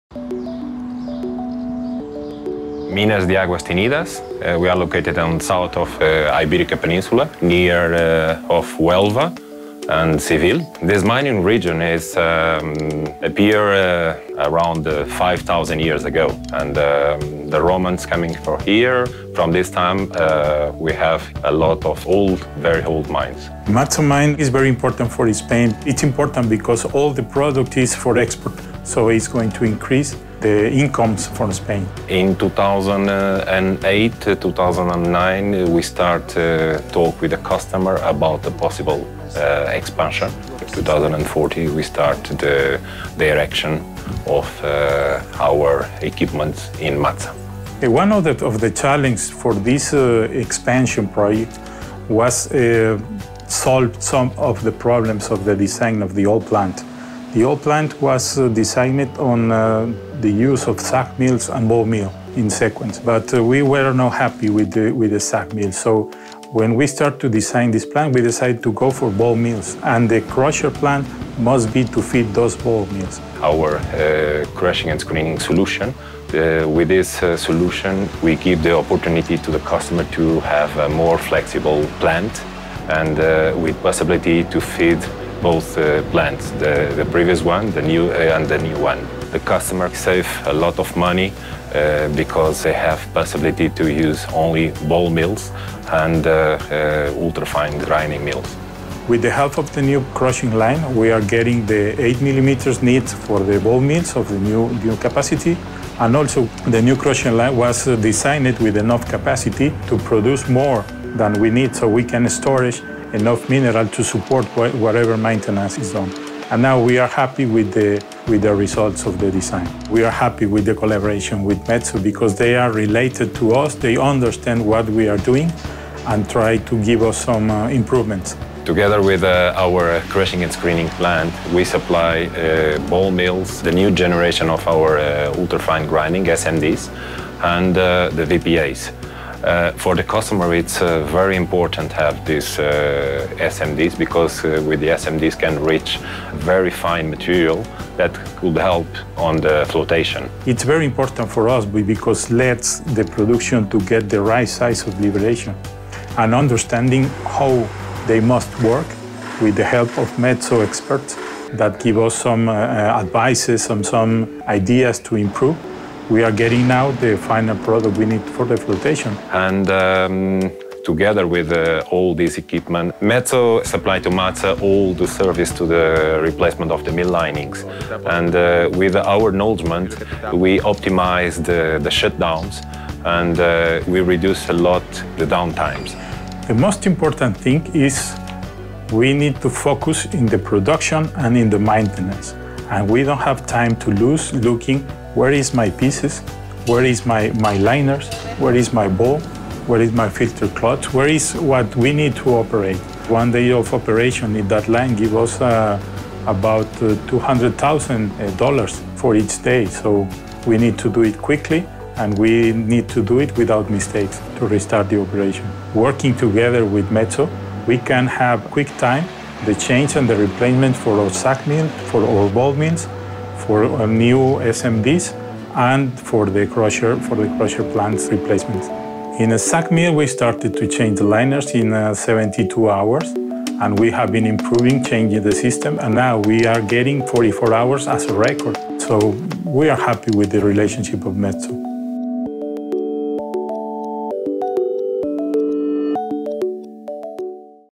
MATSA - Exploração mineira num ambiente milenar
Um processo mais inteligente e redução do tempo de inatividade em Minas de Aguas Teñidas (MATSA), Espanha.